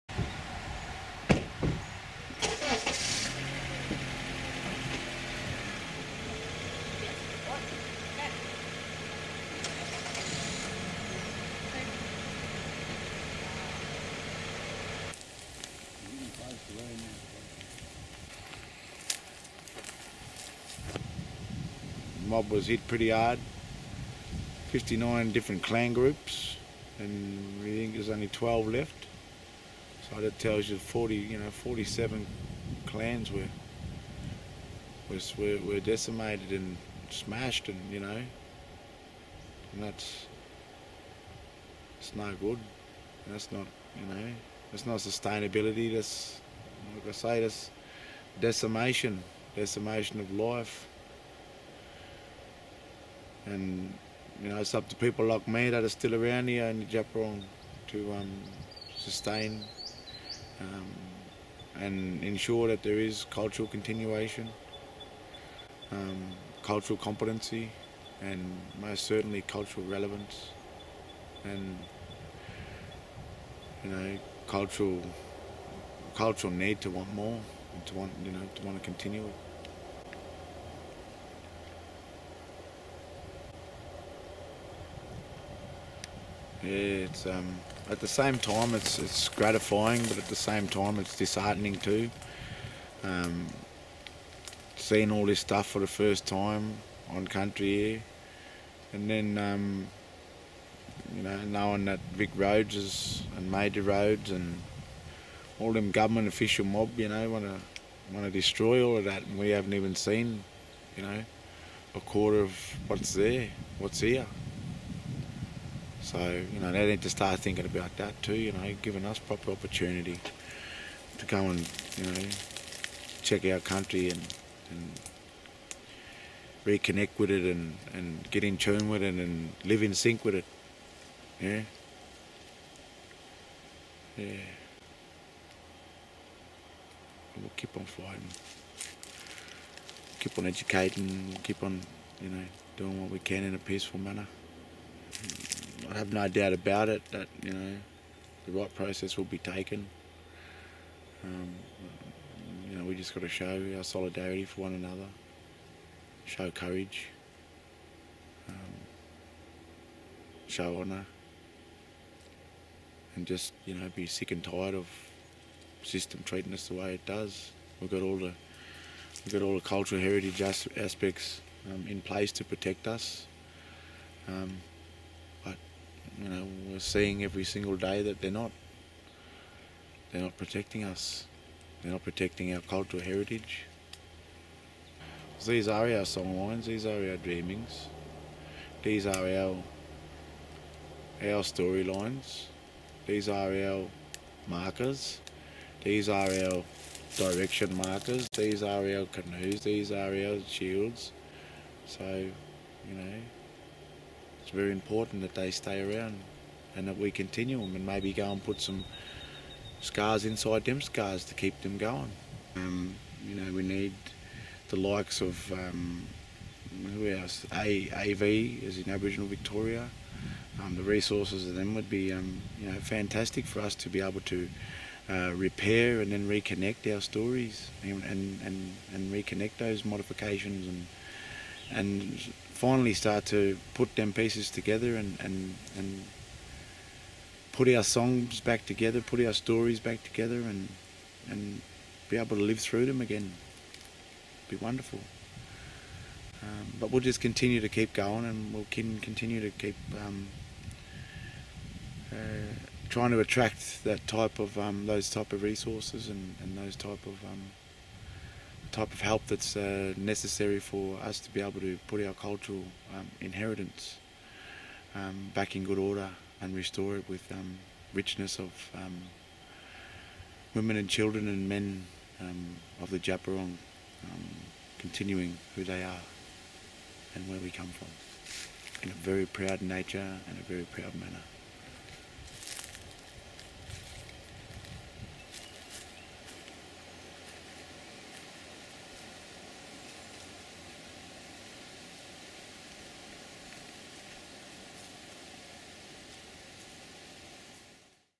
Protecting Djap Wurrung cultural heritage
Djap Wurrung man, Zellanach (DT) Djab Mara, takes us onto his country to give us further insight into why his trees and culture need protection.